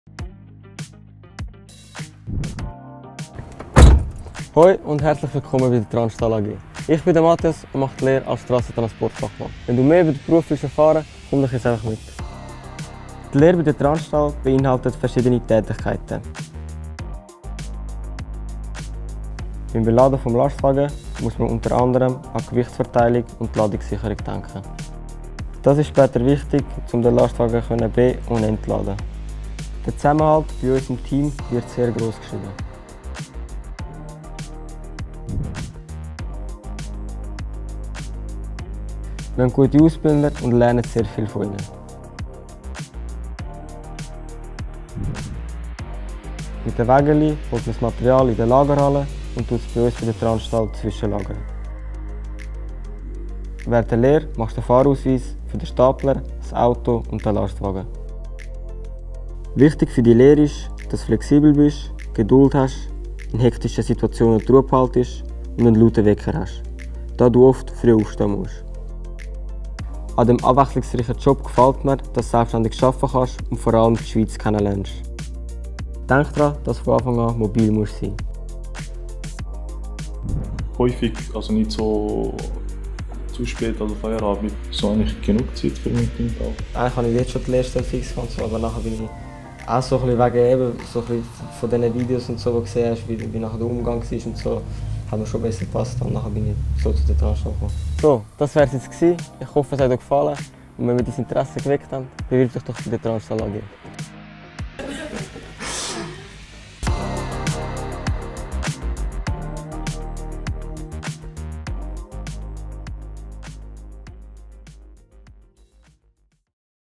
Lehre als Strassentransportfachmann/-frau bei Pestalozzi AG
Ein Einblick in den Alltag einer Lehrstelle bei Pestalozzi AG in Dietikon (ZH) als Strassentransportfachmann/-frau EFZ